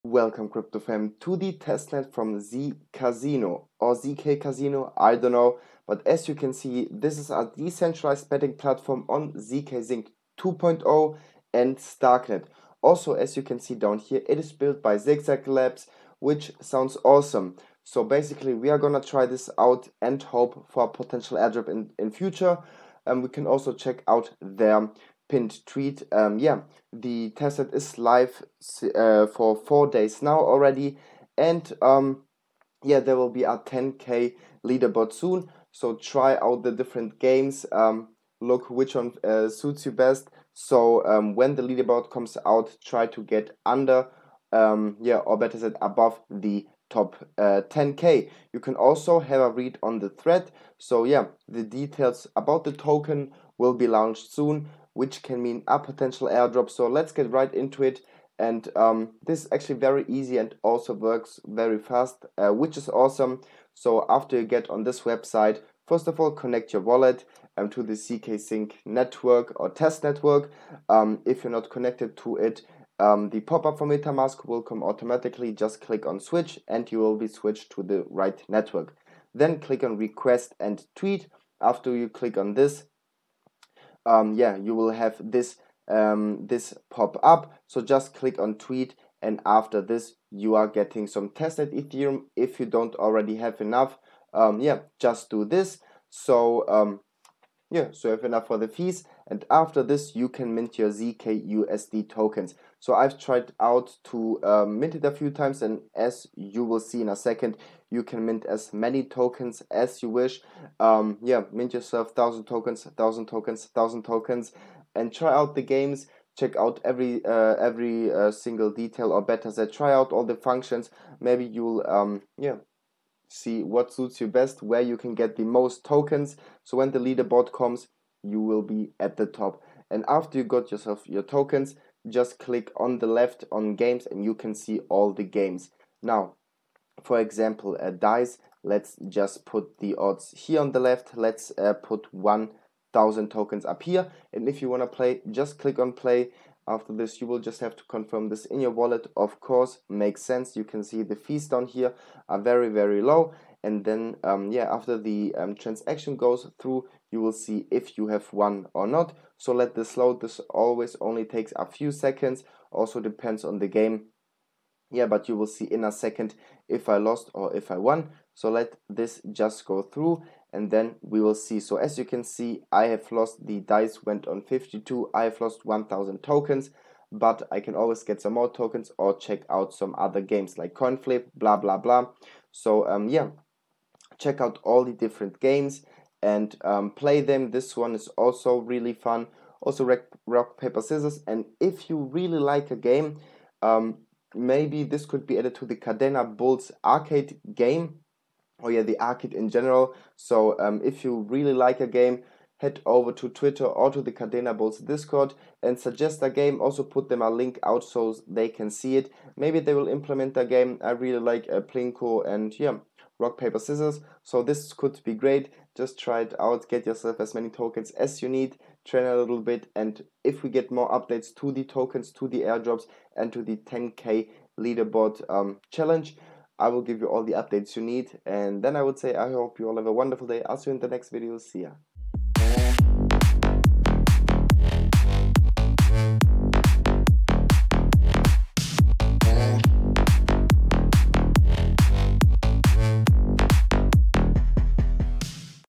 ZKasino TESTNET | potential airdrop | zkSync Airdrop
In this video I will show you how to get through the ZKasino Tetsnet. Just Play some games that's it. Have fun!
-By the way, I'm happy about feedback 😉-

⏳Timestamp⌛️

00:00 Intro
01:16 Get Faucet tokens
02:10 Play games
03:11 Outro

Also, the results may vary and you will not make any money or lose all your money.
Also counts for advertising.
Kapitel